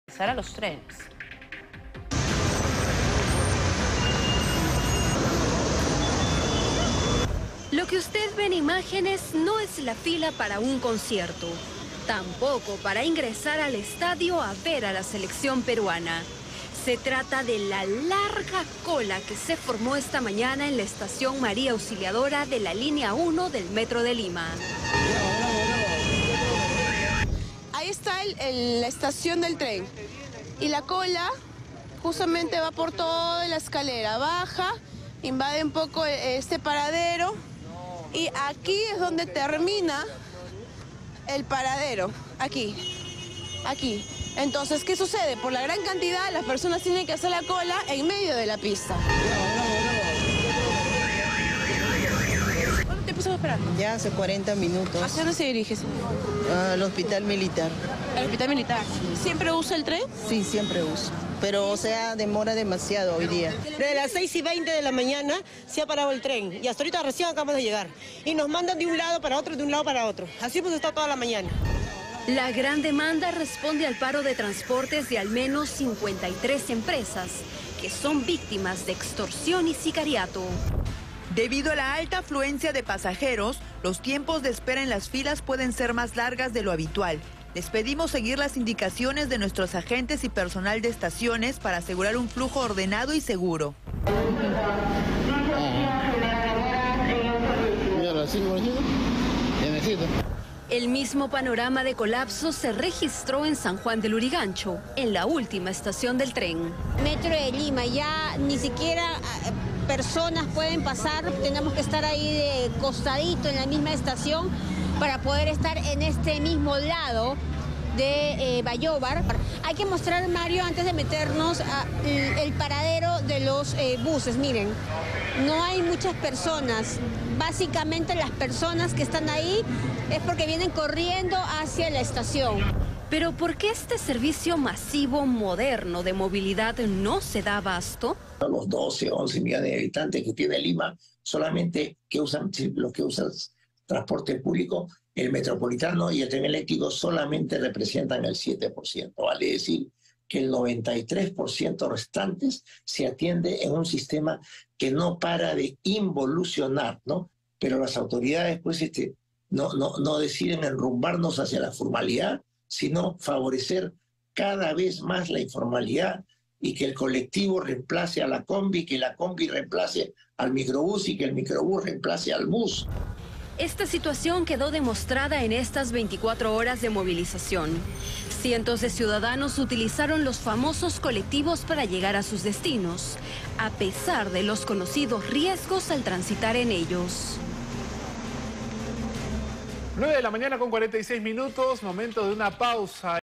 Esperaron más de 40 minutos: así fue el colapso del Metro de Lima tras paro de transportistas
Esta semana, se evidenció una alta demanda en las estaciones del Metro de Lima debido al paro de al menos 53 empresas de transportes, afectadas por la extorsión y el sicariato.

“Ya hace 40 minutos”, sostuvo una usuaria al ser consultada cuánto tiempo tenía esperando para acceder al servicio.

“De los habitantes que tiene Lima, solamente usan el transporte público, el Metropolitano y el tren eléctrico solamente representan el 7%. Vale decir que, el 93% restantes se atiende en un sistema que no para de involucionar. Pero las autoridades no deciden enrumbarnos hacia la formalidad sino favorecer cada vez más la informalidad”, sostuvo un especialista a Panamericana Televisión.

En la estación María Auxiliadora de la Línea 1, las imágenes mostraron largas filas de personas esperando ingresar, un panorama que se replicó en San Juan de Lurigancho, en la última estación del tren.

COLECTIVO INFORMALES

Ante la falta de transporte público, cientos de personas recurrieron a los colectivos informales para llegar a sus destinos, a pesar de los riesgos conocidos que implica este tipo de transporte.